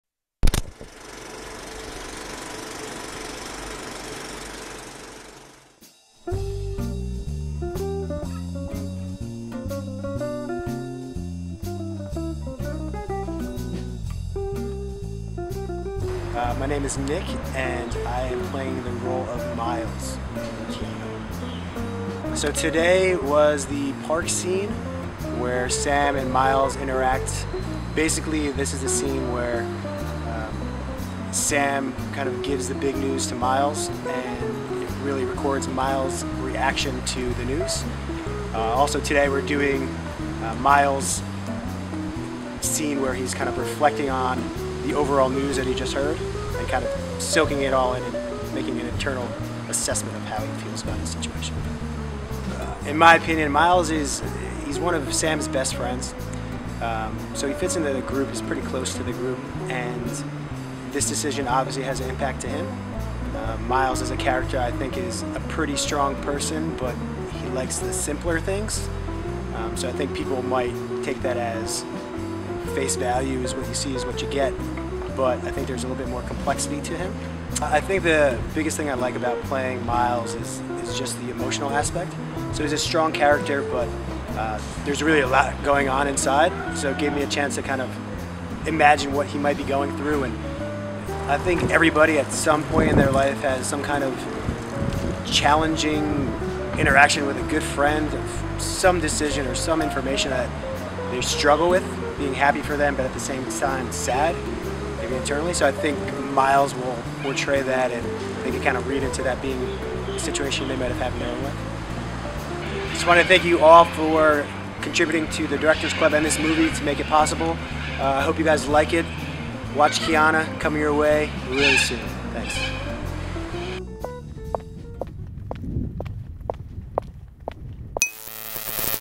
KIANA - Meet the Cast: Nick DeLillo
Interview on set with Kiana actor Nick DeLillo